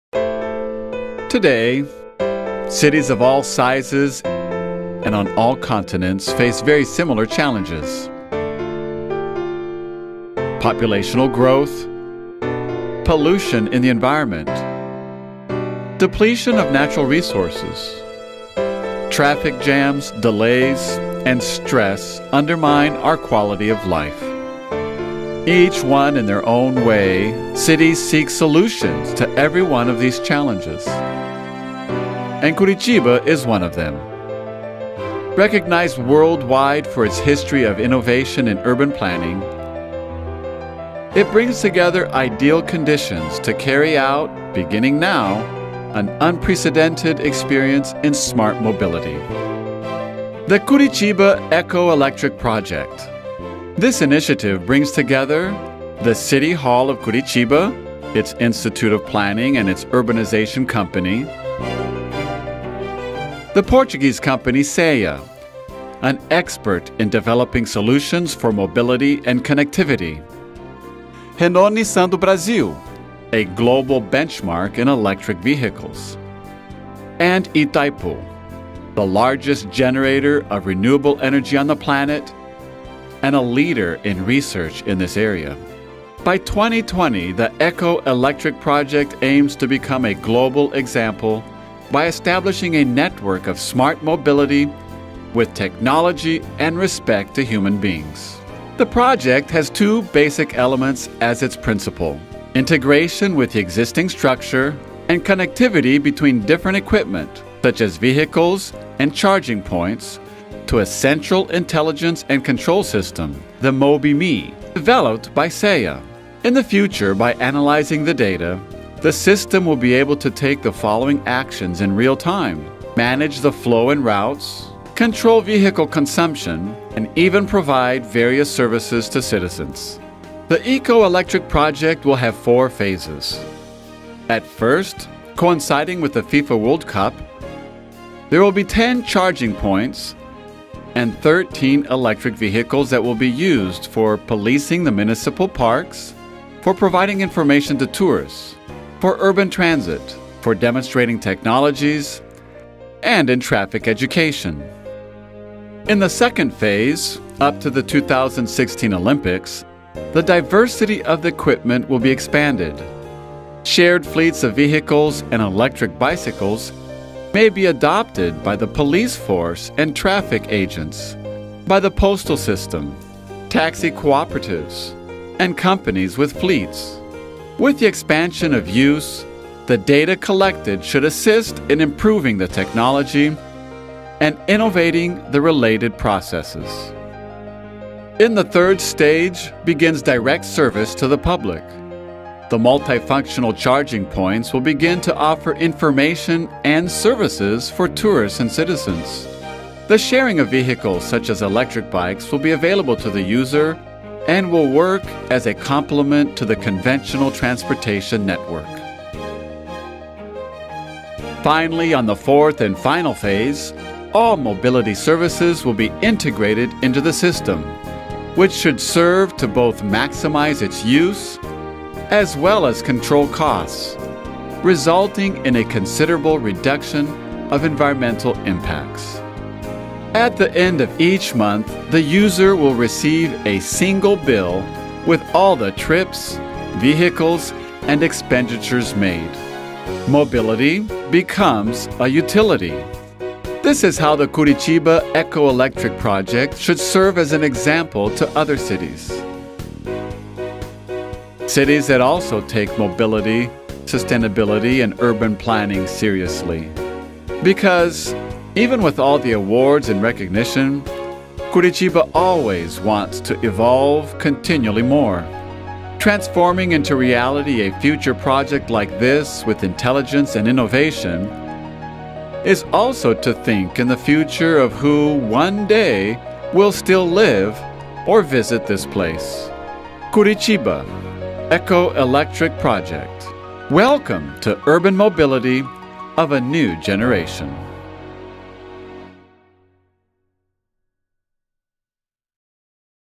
CEiiA :: Project Curitiba Eco-Eletrico by Prefeitura de Curitiba
Curitiba Eco-Elétrico – Projeto desenvolvido pelo CEiiA em parceria com a Prefeitura Municipal de Curitiba, a ITAIPU Binacional e a Aliança Renault-Nissan, que visa a criação de uma rede de mobilidade de nova geração baseada num sistema de transportes conectado, integrado e sustentável na cidade de Curitiba, com tecnologia orientada às necessidades e expectativas de mobilidade dos seus cidadãos.